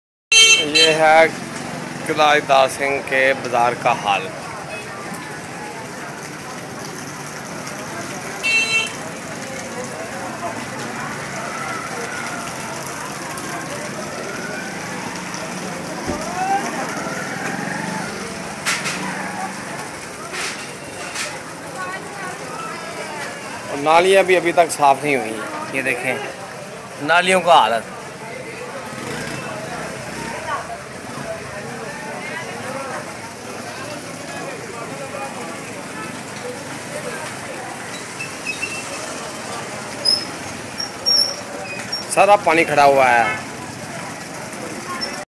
Qila Didar Singh Main Bazar Street Arraina Wali Ki Halat..31.7.2019
یہ ہے قلعہ دیدار سنگھ کے مشہور و معروف بازار گلہ ارائیناں والہ کی حالت زار گذشتہ گیارہ ماہ میں ایک لاکھ ابادی رکھنے والے اس شہر میں کوئی بھی ترقیاتی کام نہیں ہوا میاں نواز شریف کے دور میں جو بھی فنڈز ملتا تھا وہ ٹاؤن کمیٹی کے چیئرمین کھا جاتے تھے۔۔۔